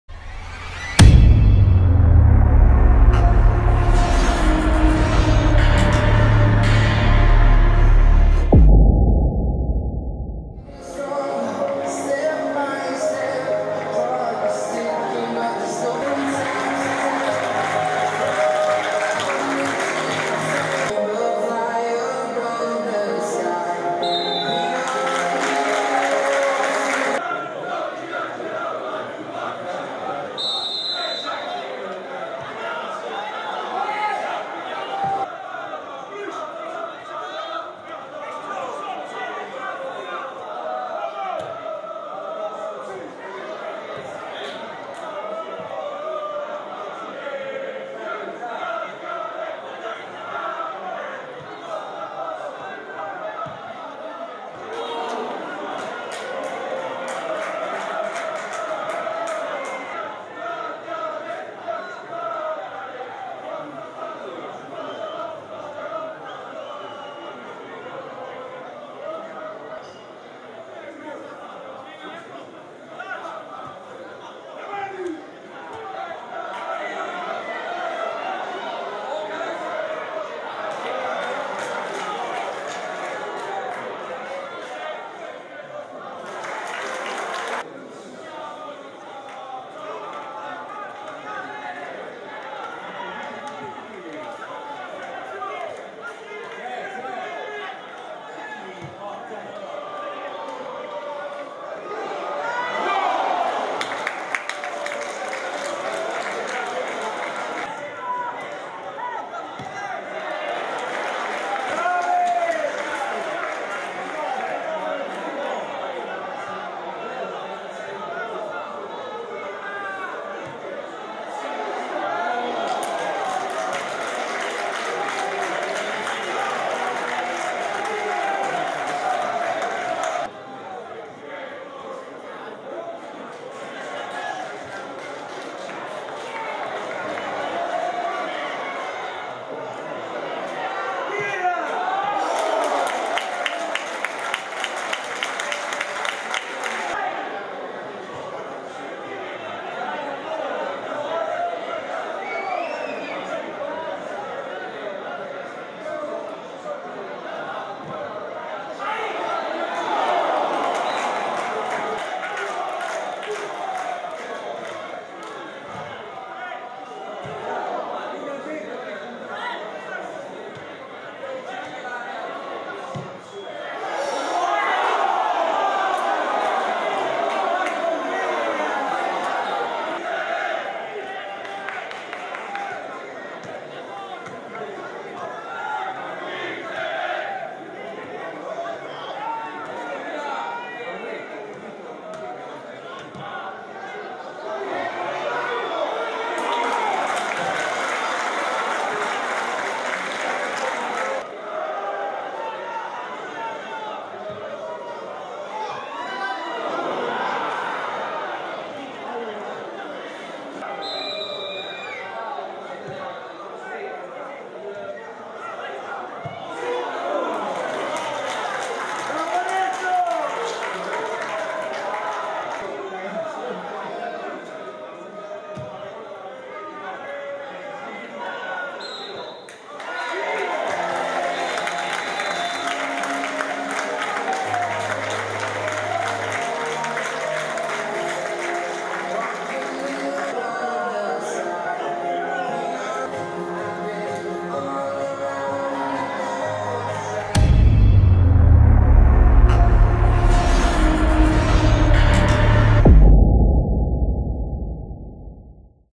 Fiorenzuola-Padova 0-0 Highlights || 25° Giornata Serie C 2021/2022
FIORENZUOLA - PADOVA 0-0

FIORENZUOLA : Battaiola; Fracassini, Ferri, Varoli, Guglieri; Nelli (32° st Oneto), Stronati (20° st Fiorini), Piccinini; Blue Mamona (38° st Sartore), Mastroianni (38° st Arrondini), Zunno (32° st Bruschi). In panchina: Burigana, Dimarco, Potop, Danovaro, Ghisolfi, Cavalli, Giani. Allenatore: Tabbiani.

PADOVA : Donnarumma; Germano, Ajeti, Monaco, Curcio (31° st Gasbarro); Saber (31° st Bifulco), Ronaldo, Della Latta; Chiricò (12° st Jelenic), Ceravolo (20° st Nicastro), Terrani (12° st Santini). In panchina: Vannucchi, Pelagatti, Valentini, Dezi, Busellato, Kirwan. Allenatore: Pavanel

Arbitro: Del Rio di Reggio Emilia